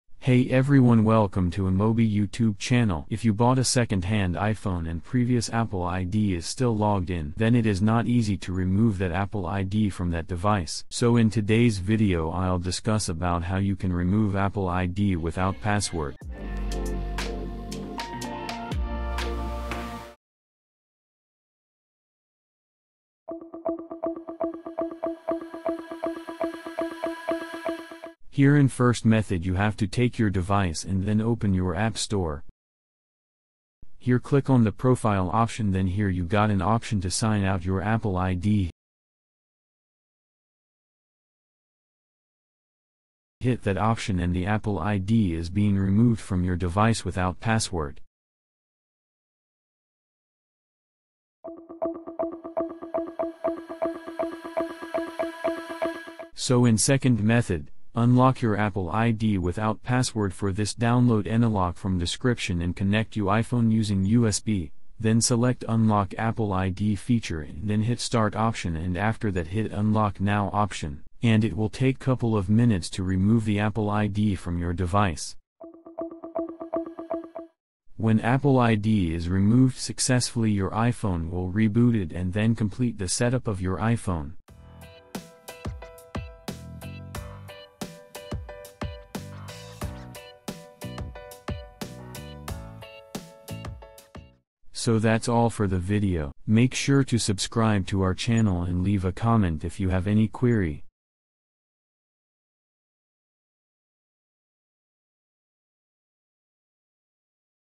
[2 Ways] How to Sign Out of Apple ID without Password | iOS 16.4
Are you looking to sign out of your Apple ID but not sure how to do it? Don't worry, in this easy-to-follow guide, we will show you how to sign out of your Apple ID on your iPhone without a password.

Whether you want to sign out of your Apple ID because you're sharing your device with someone else, or you're planning to sell it or give it away, this guide will help you do it quickly and easily. By the end of this video, you will know exactly how to sign out of your Apple ID on all your devices, and you'll have peace of mind of knowing your data is secure.
00:00 Intro
00:25 Sign out of Apple ID from Apple Store
00:54 Sign out of Apple ID without Password
Check the article to get more tips about removing the device from Apple ID
Check more videos on our channel: